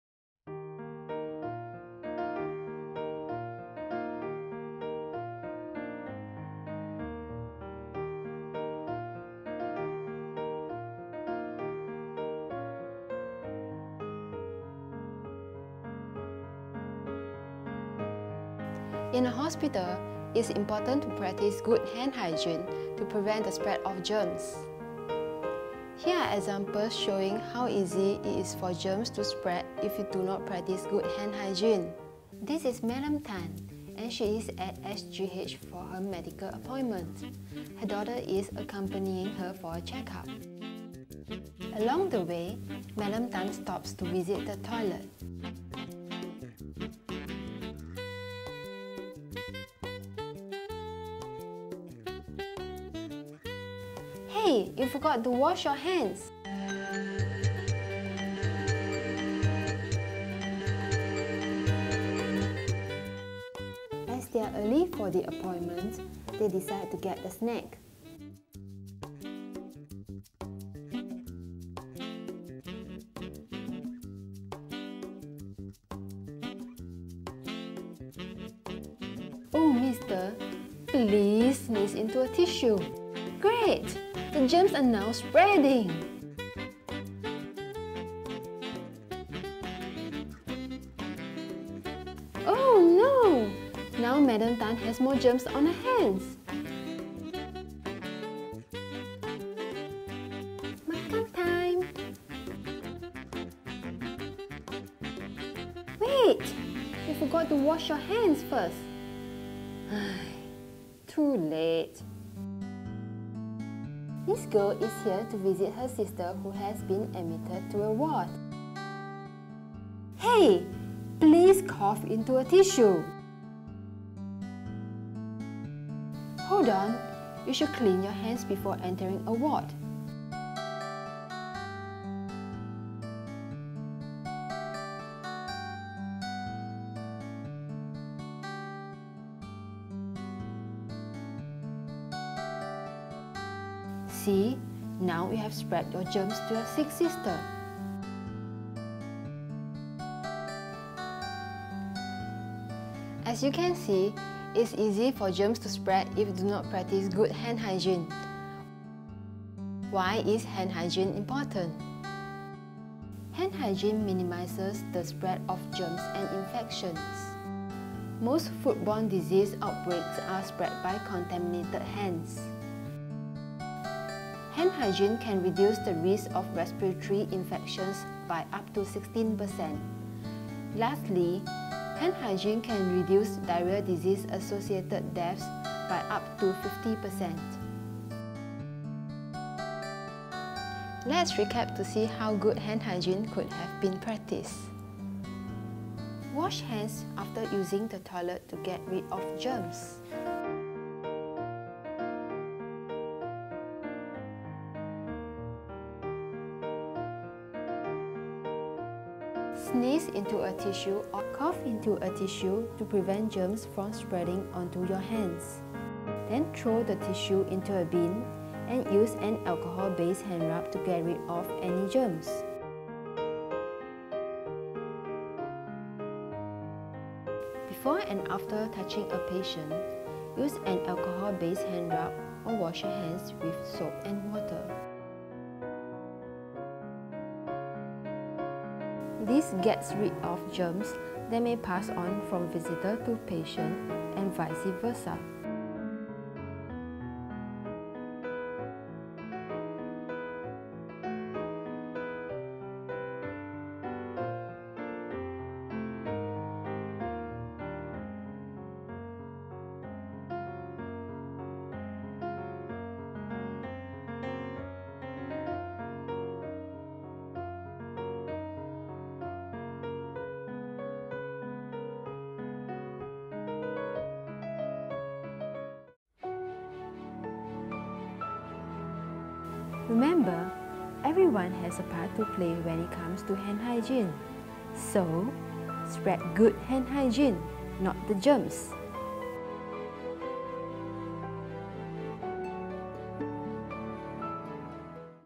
What happens if you neglect hand hygiene? - Singapore General Hospital
What happens if you neglect hand hygiene? Practicing good hand hygiene ettiquette is cruical to prevent the transmission of harmful germs.

You will see examples of how easily germs can be spread from one party to another. Find out how you can do your part to prevent the spread of germs and infections. Watch on for essential tips to practice good hand hygiene such as covering coughs and sneezes.

Remember, spread good hand hygiene, not the germs!

Chaptering
00:00 Introduction
00:26 Examples of how easy germs can spread
00:35 Scenario #1
01:11 Scenario #2
01:59 Scenario #3
02:39 Why is hand hygiene important?
03:15 Good hand hygiene practices
04:07 7 Steps of Hand Hygiene
04:41 Outro